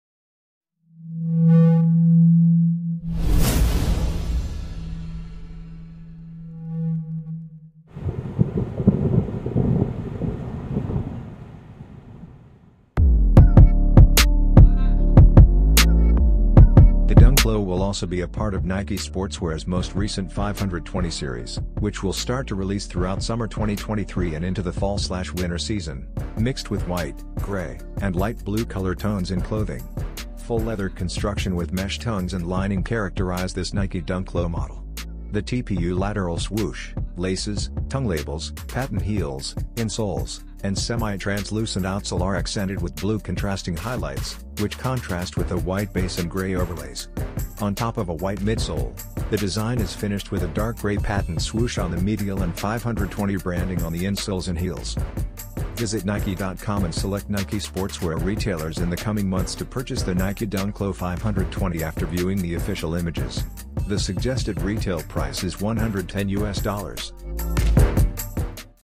520 2023 Nike Dunk Low DETAILED LOOK + PRICE
Nike Dunk Low
520
2023

FOR ADULTS
LIFESTYLE

Style Code: FN3433-141
Release Date: FALL / WINTER 2023 TBA
Price: US$110